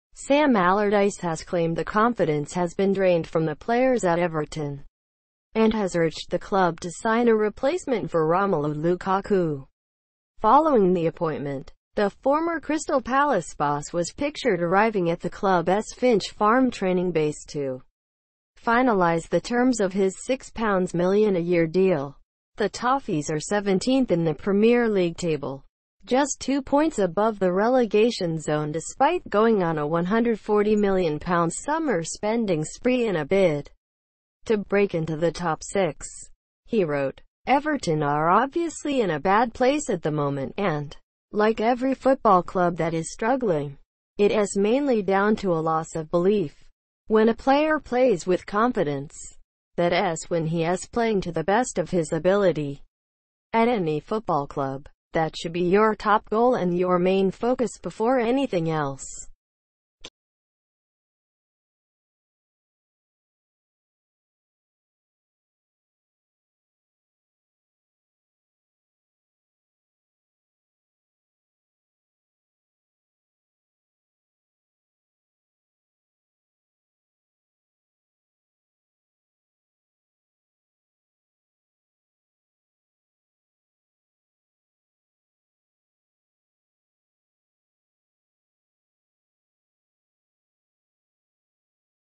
Everton players' 'confidence has been drained' - Allardyce ‍
[ Sam Allardyce has claimed the  confidence has been drained from the players  at Everton — and has urged the club to sign a replacement for Romelu Lukaku.  ,   ,  Following the appointment, the former Crystal Palace boss was pictured arriving at the club s Finch Farm training base  to finalise the terms of his £6milllion-a-year deal. ,  The Toffees are 17th in the Premier League table, just two points above the relegation zone despite going on a £140m summer spending spree in a bid to break int